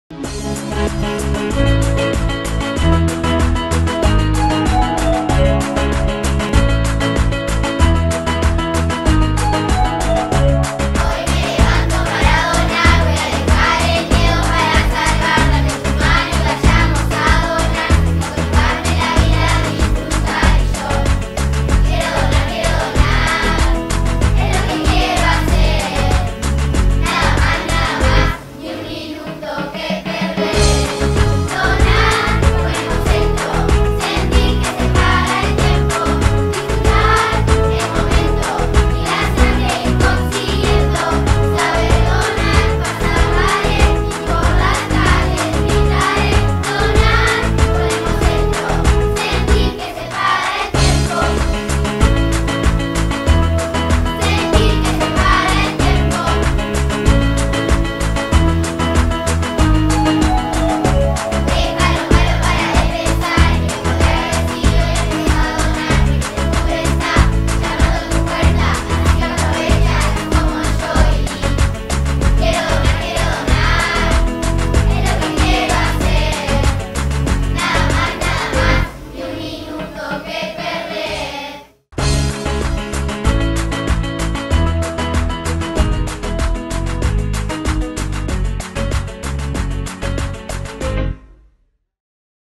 Convocatoria alumnos del Colegio Bilingüe de Minas a su Jornada de Donación de Sangre.-
Convocatoria a Jornada de Donación de Sangre del Programa Educativo el Viernes 4 de Noviembre de 08:30 a 14:30 hrs. en el Hemobus en el centro educativo.